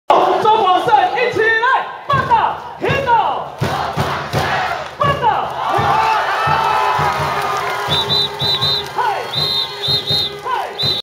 150614 - 台南 - 統一獅 - 1下 - 周廣勝 - 應援 (1)
第 5 棒 三壘手 周廣勝：左外野方向一壘安打，一二壘有人